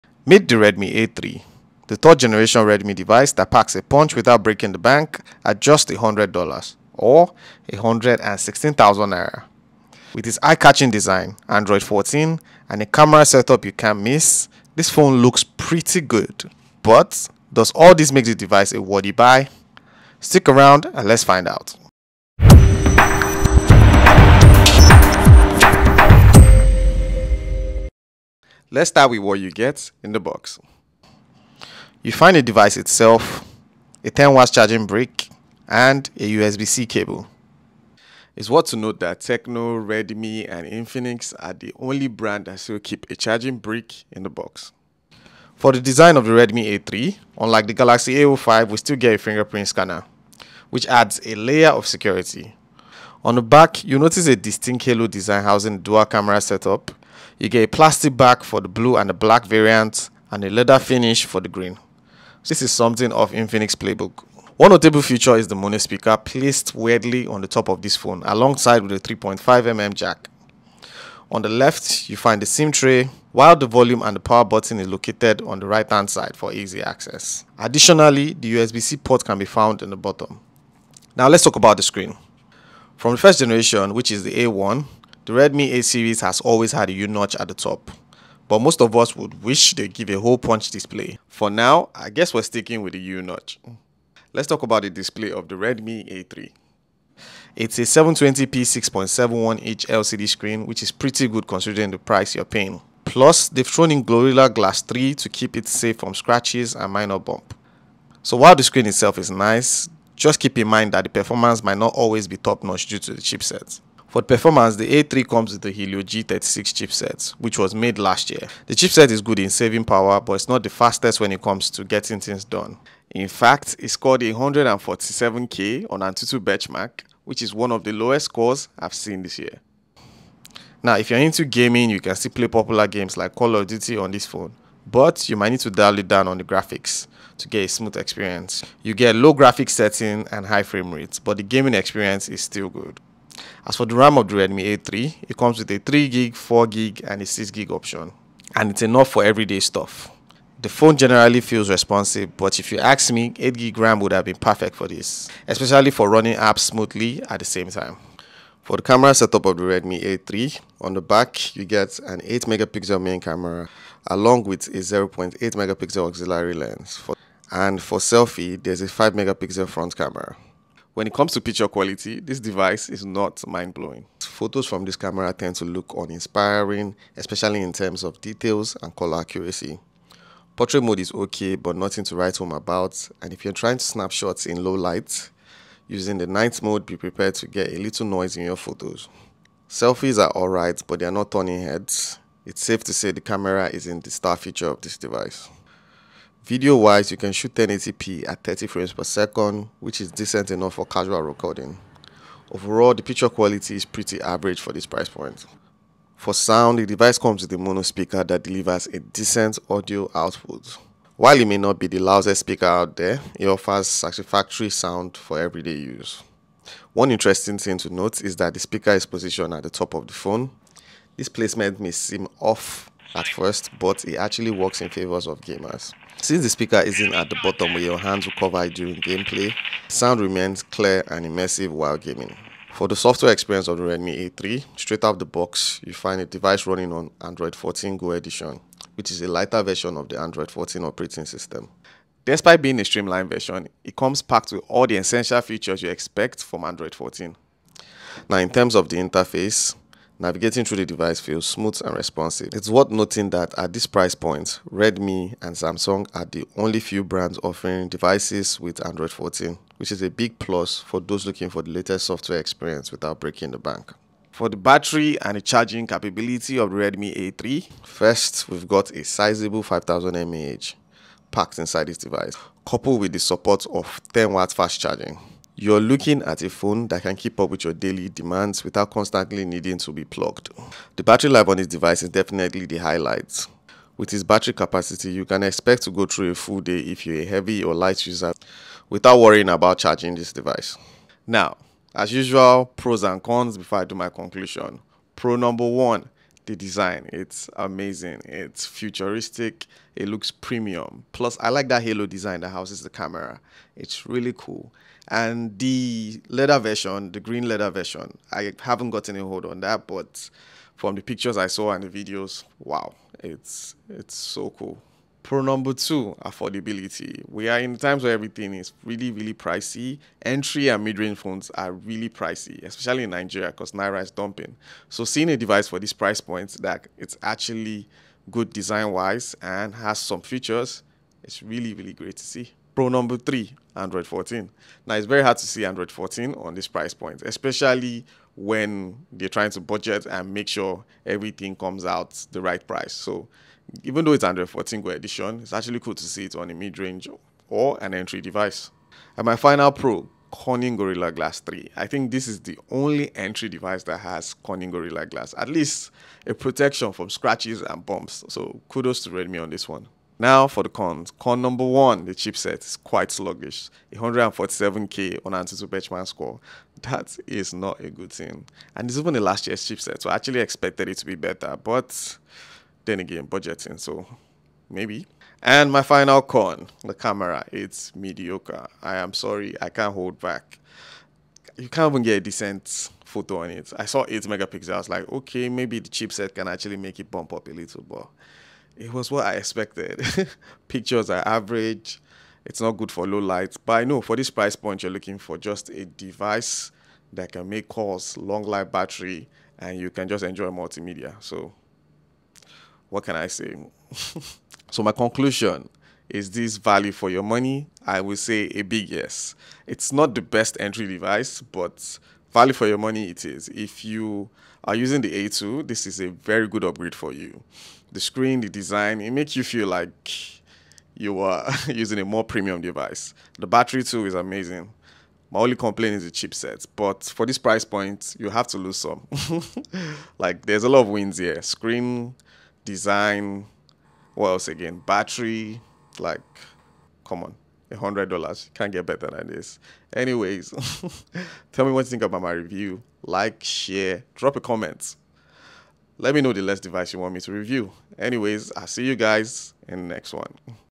Redmi A3 Unboxing and Review
Welcome to our detailed review of the Redmi A3, where we explore its features, performance, camera quality, battery life, and more to determine if it truly stands out as the top budget-friendly device in the market. In this video, we'll cover all aspects of the Redmi A3, including its specifications, design, display performance, software experience, and overall value for money. Whether you're considering buying the Redmi A3 or simply interested in learning more about its capabilities, this review will equip you with all the necessary insights to make an informed decision.